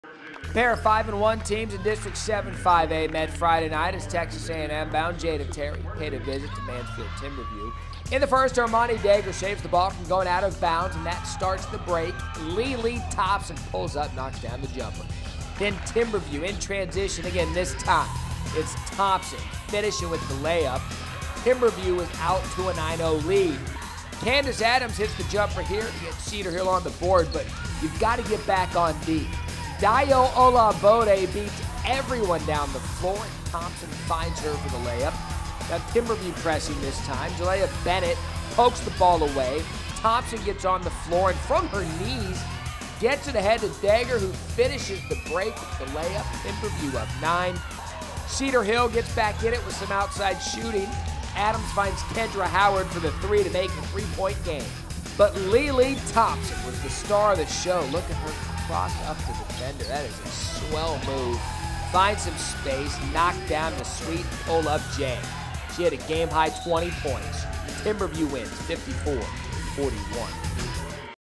Cedar Hill Lady Longhorns vs. Timberview Lady Wolves - 2013 Basketball - Week 2
A pair of 5-1 teams in district 7-5A met Friday night as Texas A&M-bound Jada Terry paid a visit to Mansfield Timberview...In the first, Aurmani Degar saves the ball from going of bounds and that starts the break... Leelee Thompson pulls up and knocks down the jumper... Then Timberview in transition again, this time it's Thompson finishing with the lay-up... Timberview was out to a 9-0 lead...Candice Adams hits the jumper here to get Cedar Hill on the board, but you've gotta get back on D!... Dayo Olabode  beats everyone down the floor and Thompson finds her for the lay-up...Now, Timberview pressing, Jalea Bennett pokes the ball away, Thompson gets on the floor and from her knees gets it ahead to Degar who finishes the break with the lay-up... Timberview up 9 Cedar Hill gets back in it with some outside shooting... Adams finds Kendra Howard for the three to make it a three-point game...But Leelee Thompson was the star of the show... Look at her cross up her defender and find some space to knock down the sweet pull-up jumper... She had a game-high 20 points and Timberview wins 54-41.